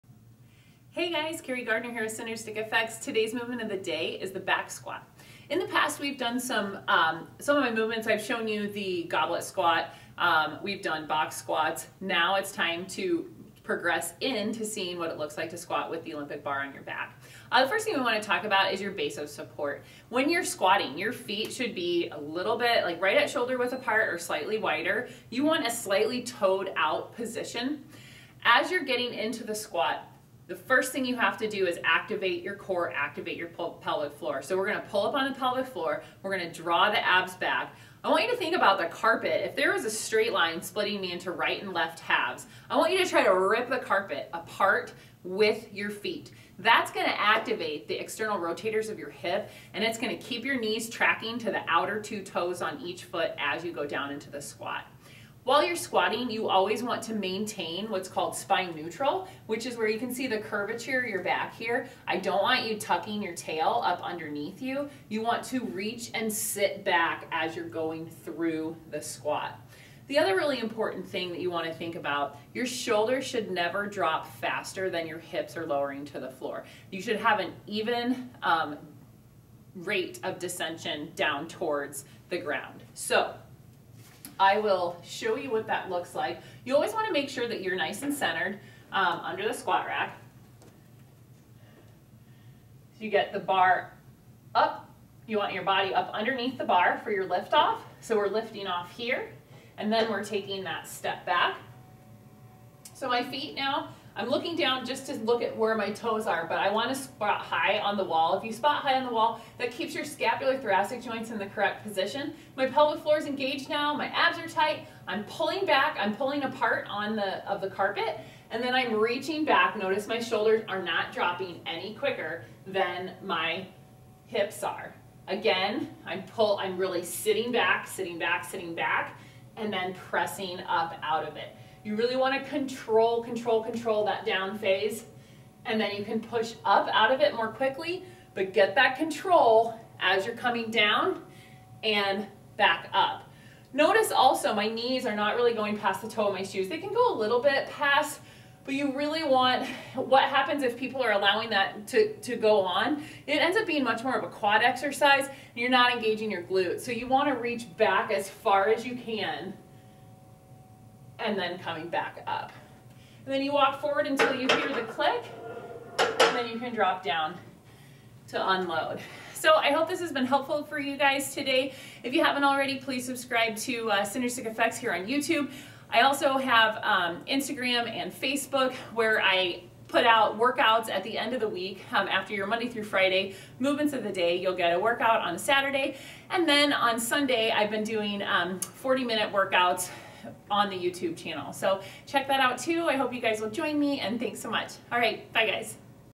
Back Squats
Today's "Movement of the Day" is the Back Squat. Back squats are an excellent posterior chain, multi-joint lower body exercise targeting the gluteus maximus, quadriceps femoris, adductor magnus, and soleus. The back squat also isometrically loads the erector spinae as well as the the abdominals musculature while using others as stabilizers such as the hamstrings, internal and external obliques, deep external rotators of the hip and the gastrocnemius.